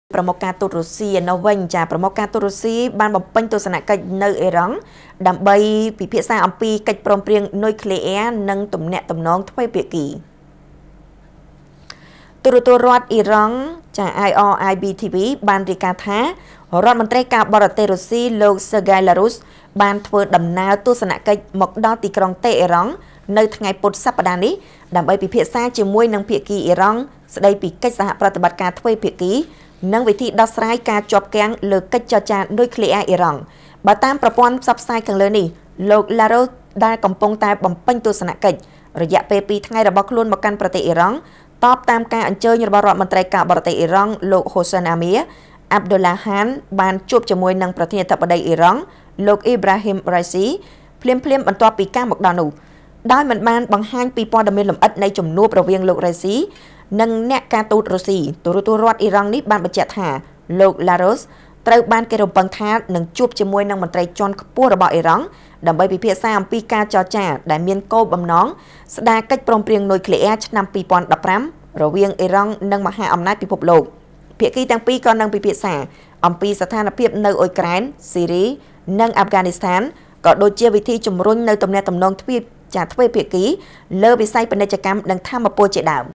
ប្រមុខការទូតរុស្ស៊ីបំពេញទស្សនកិច្ចនៅអ៊ីរ៉ង់ ដើម្បីពិភាក្សាអំពីកិច្ចព្រមព្រៀងនុយក្លេអ៊ែរ
ប្រមុខការទូតរុស្ស៊ីបំពេញទស្សនកិច្ចនៅអ៊ីរ៉ង់ ដើម្បីពិភាក្សាអំពីកិច្ចព្រមព្រៀងនុយក្លេអ៊ែរ និងទំនាក់ទំនងទ្វេភាគី